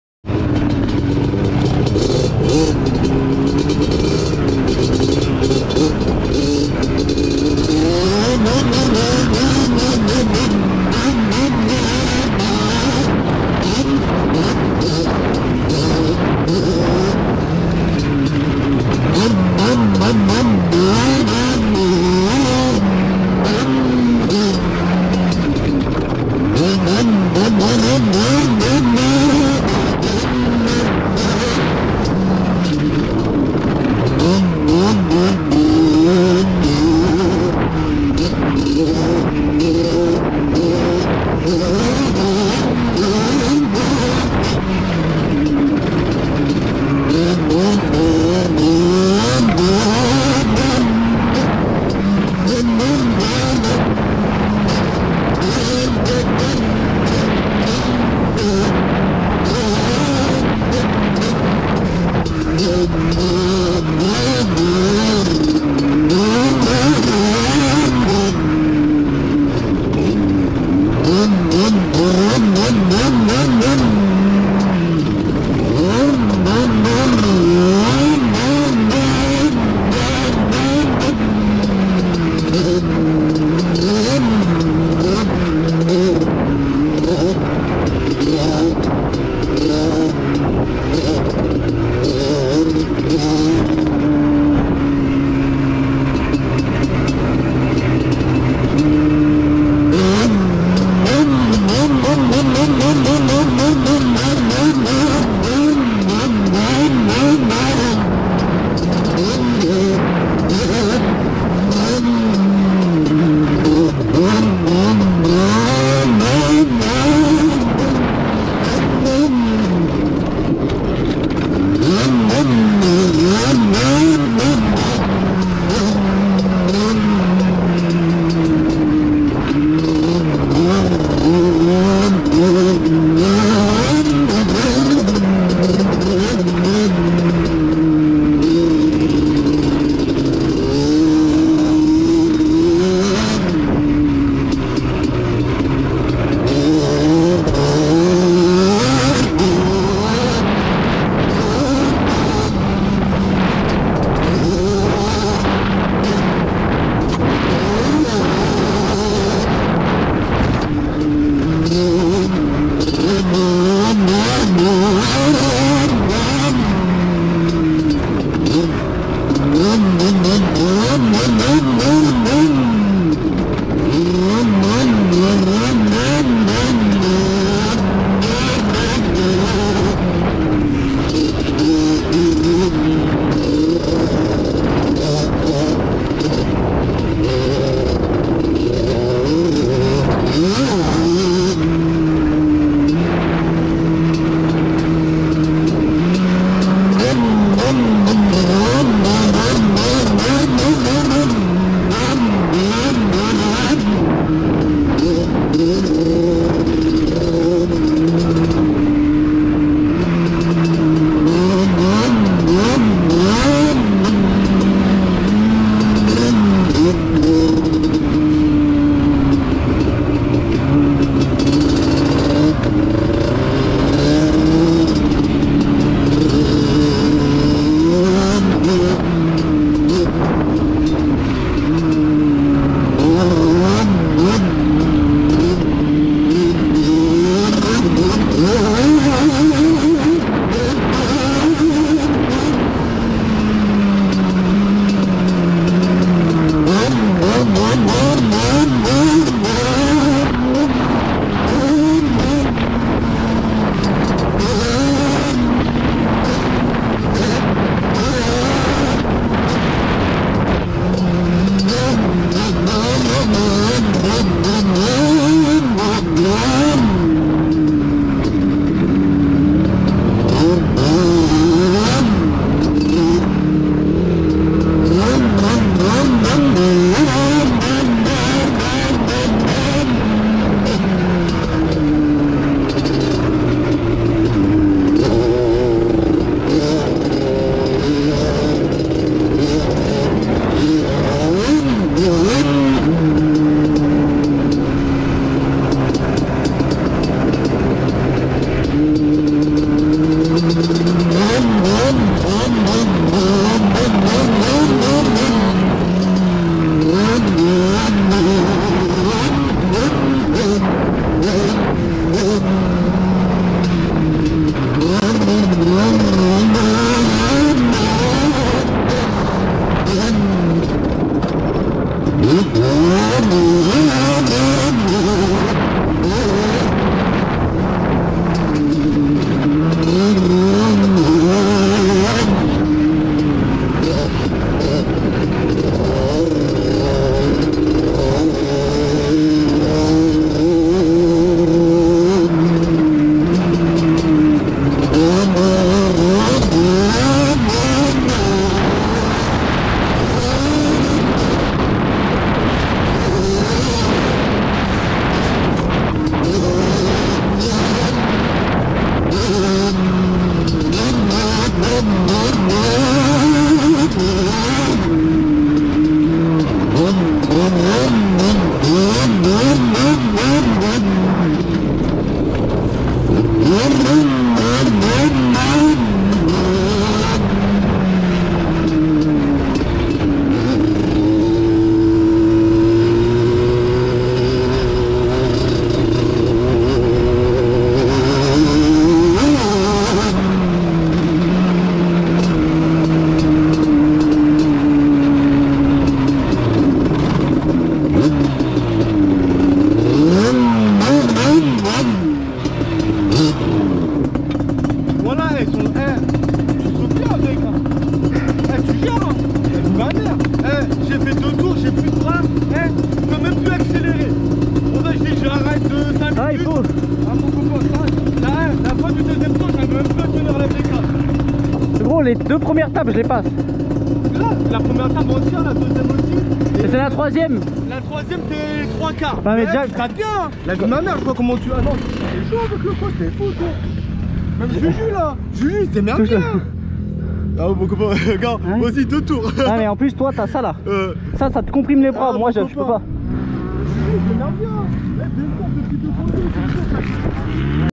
350 banshee sur terrain de cross
Ma première en quad sur terrain de cross a Londinières